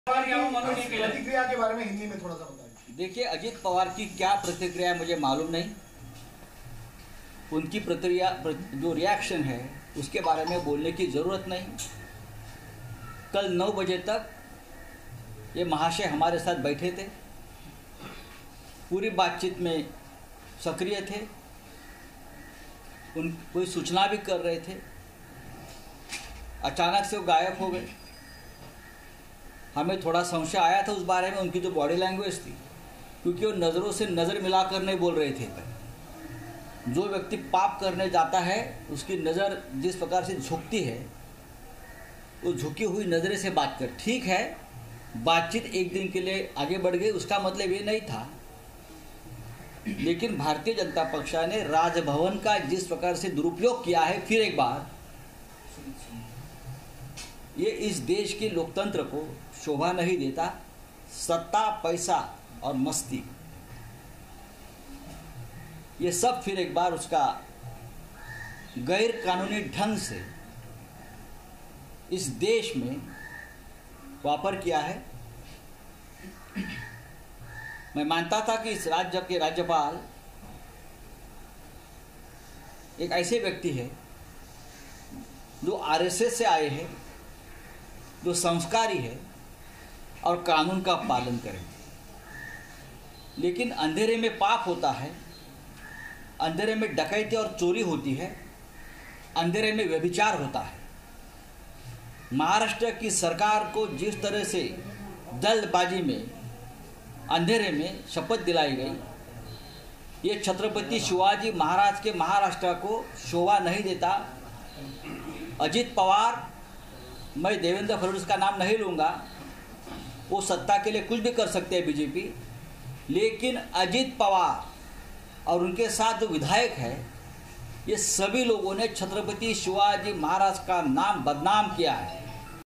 Sanjay Raut ने बीजेपी, अजित पवार और राज्यपाल पर साधा निशाना
महाराष्ट्र की सियासी उलटफेर पर संजय राउत ने बीजेपी, अजित पवार और महाराष्ट्र के राज्यपाल पर साधा निशाना. देखिए, शिवसेना के संजय राउत क्या कहा...

About Channel